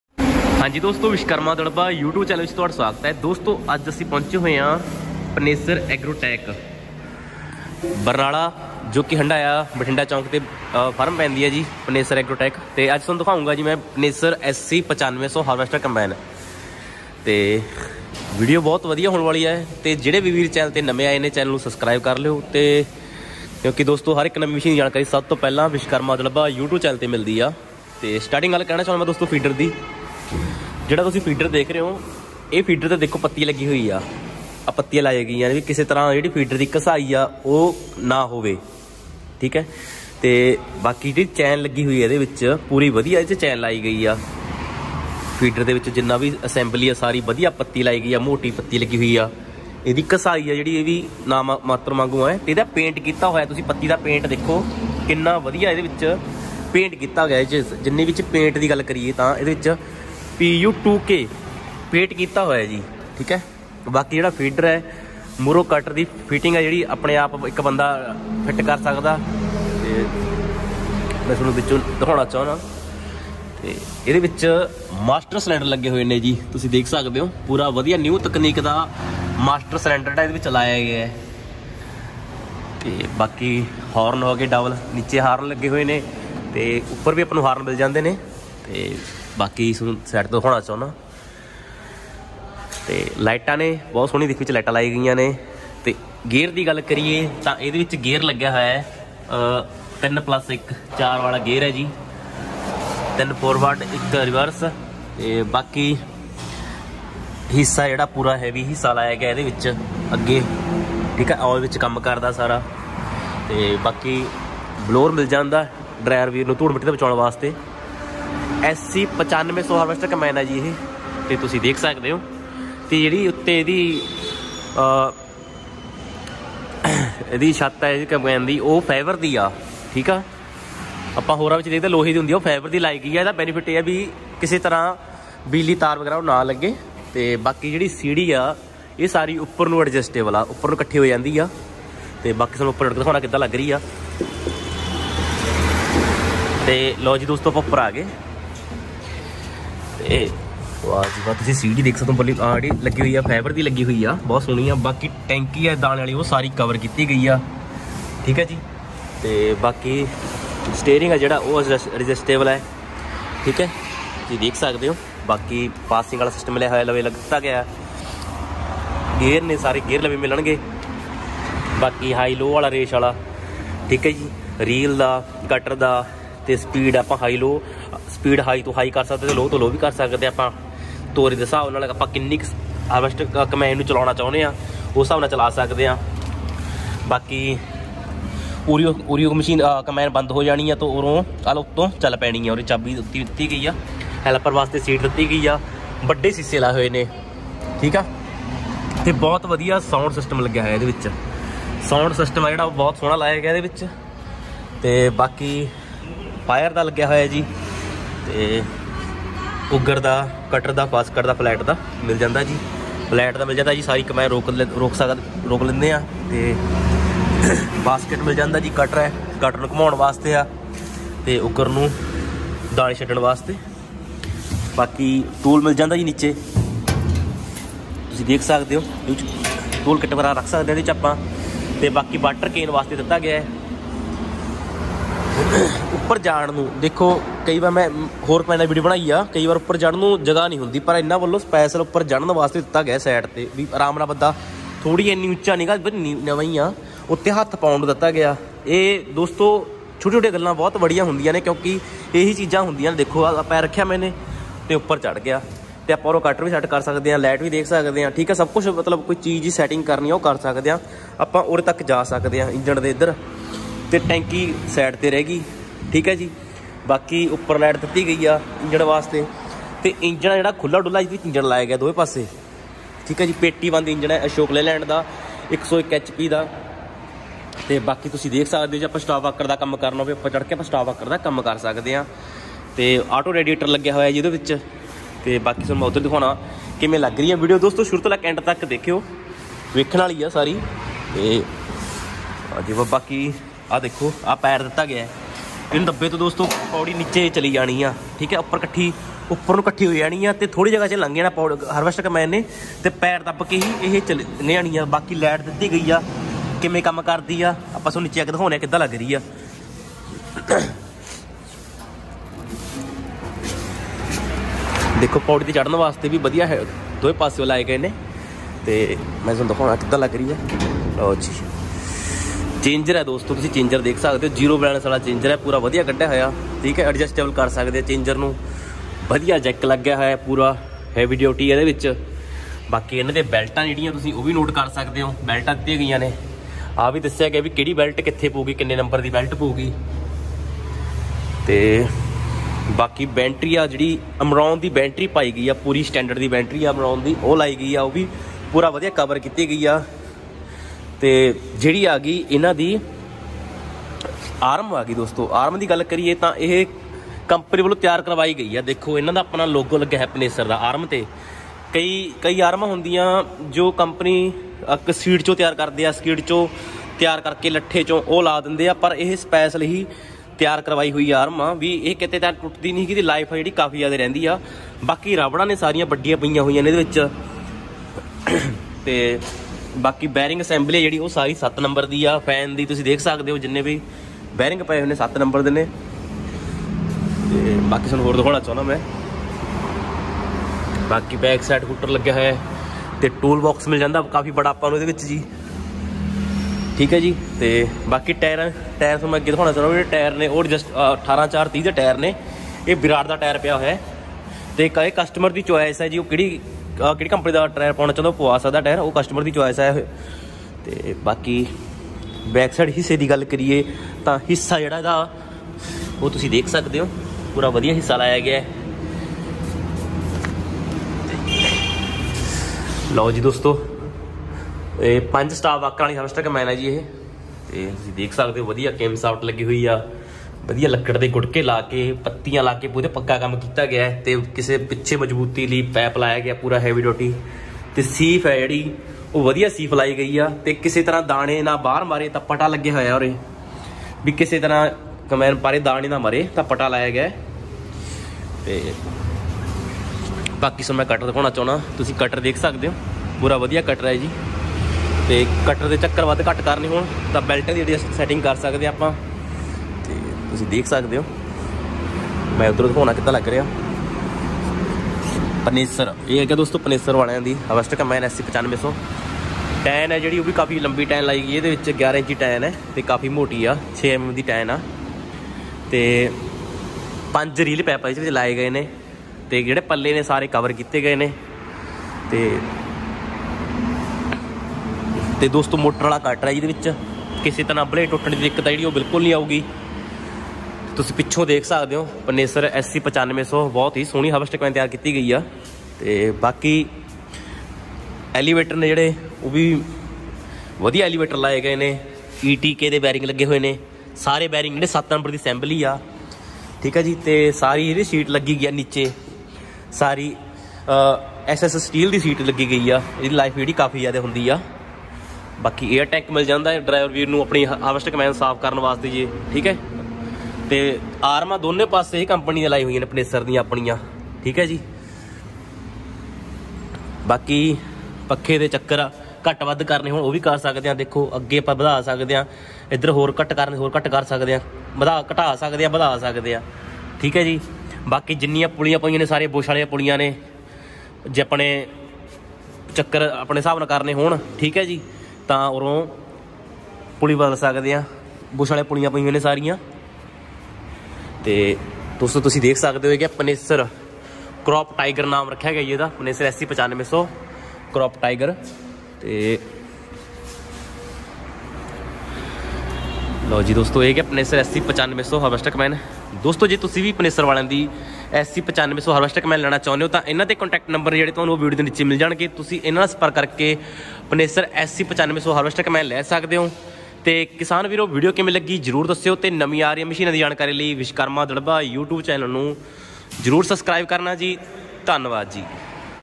Panesar SC 9500 Harvester Combine New Model 2022 Full Review With Price
camera :- Ramandeep Kaur

Editing:- Jashan

Helpline:- E cat Institute Dirba

Connect us on :- Amritdhiman26 -